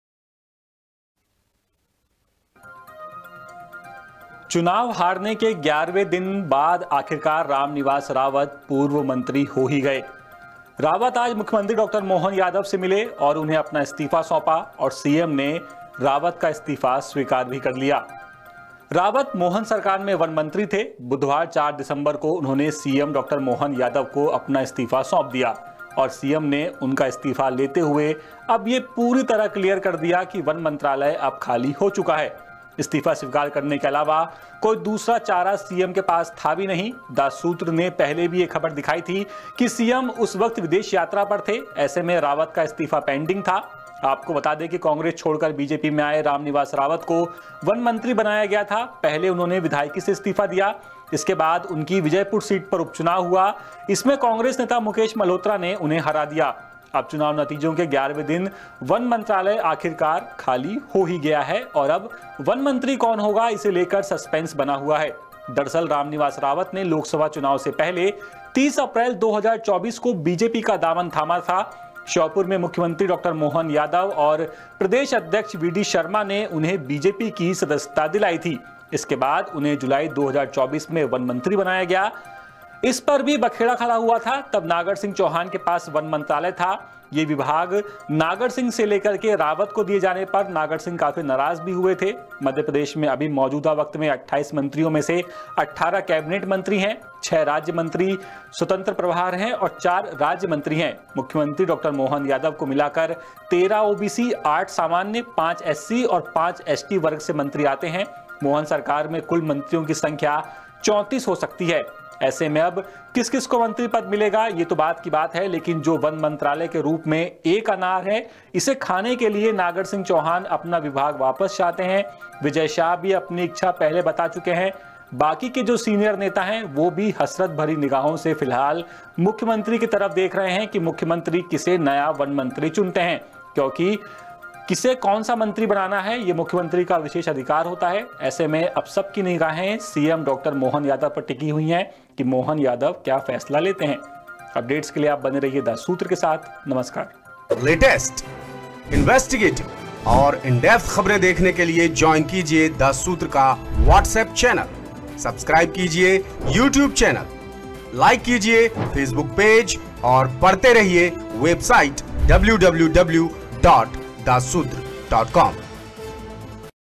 चुनाव हारने के 11 दिन बाद Ramniwas Rawat ने मंत्री पद से दिया इस्तीफा | CM Mohan Yadav से की मुलाकात
चुनाव हारने के 11वें दिन बीजेपी नेता रामनिवास रावत ने मंत्री पद से इस्तीफा दे दिया। रावत मोहन सरकार में वन मंत्री थे। बुधवार, 4 दिसंबर को उन्होंने सीएम डॉ.मोहन यादव को अपना इस्तीफा सौंपा। ---
madhya pradesh, mp news, mohan yadav, cm mohan yadav, ramniwas rawat,